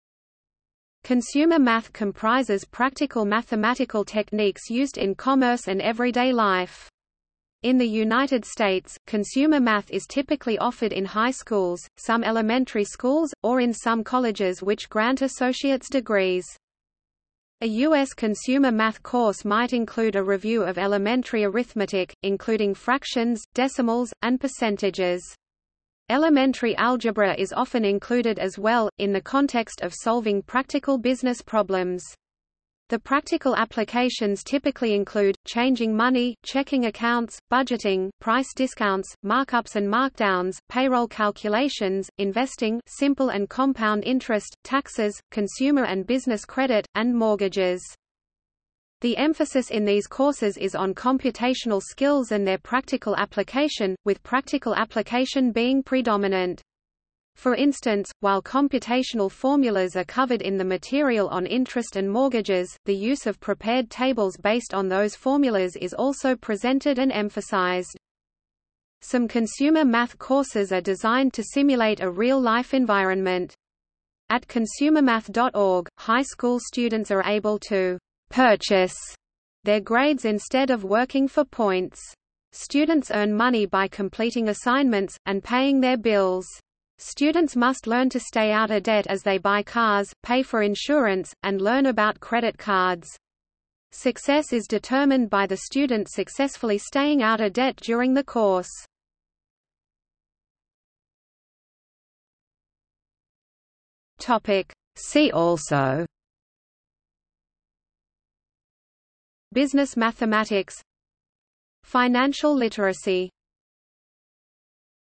Consumer math | Wikipedia audio article
00:01:40 See also

- Socrates

SUMMARY
Consumer math comprises practical mathematical techniques used in commerce and everyday life.  In the United States, consumer math is typically offered in high schools, some elementary schools, or in some colleges which grant associate's degrees.
A U.S. consumer math course might include a review of elementary arithmetic, including fractions, decimals, and percentages. Elementary algebra is often included as well, in the context of solving practical business problems. The practical applications typically include: changing money, checking accounts, budgeting, price discounts, markups and markdowns, payroll calculations, investing (simple and compound interest), taxes, consumer and business credit, and mortgages.
The emphasis in these courses is on computational skills and their practical application, with practical application being predominant. For instance, while computational formulas are covered in the material on interest and mortgages, the use of prepared tables based on those formulas is also presented and emphasized.
Some consumer math courses are designed to simulate a real life environment.  At ConsumerMath.org, high school students are able to "purchase" their grades instead of working for points.  Students earn money by completing assignments, and paying their bills.  Students must learn to stay out of debt as they buy cars, pay for insurance, and learn about credit cards.  Success is determined by the student successfully staying out of debt during the course.